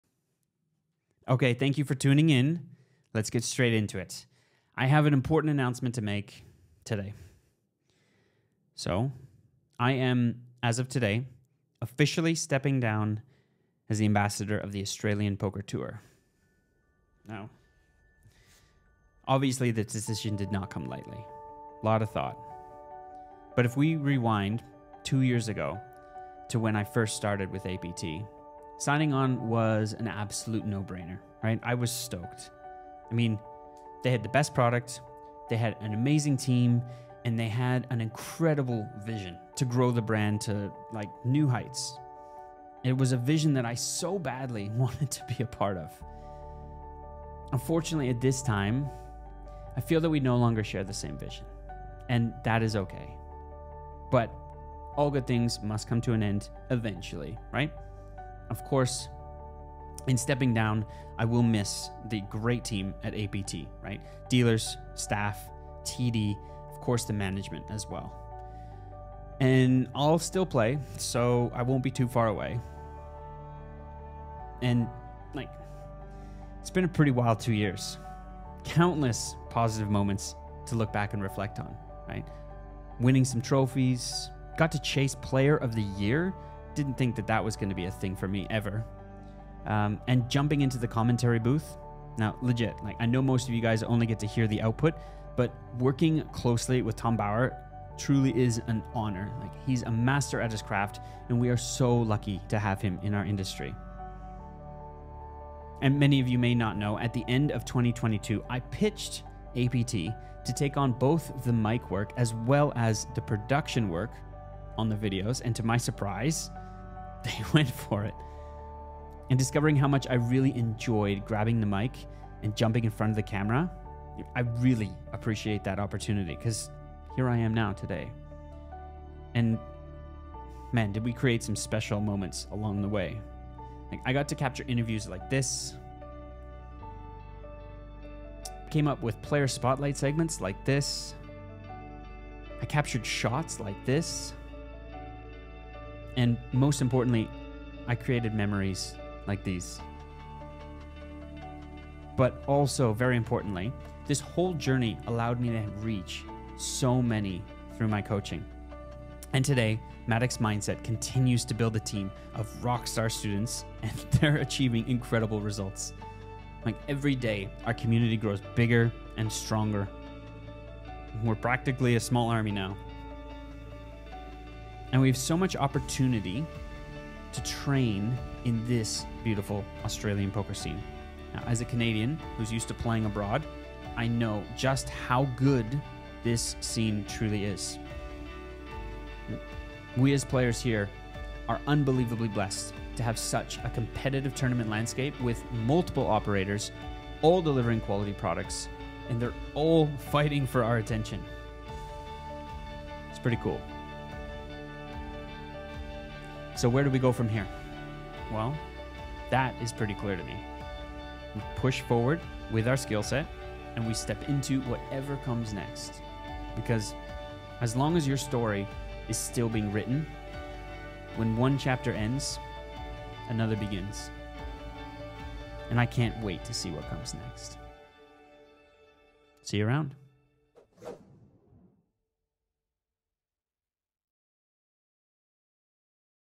It's time for a major change
For the last two years, I have been serving as the Ambassador for the Australian Poker Tour. Alas, after recent developments, I have decided to step down.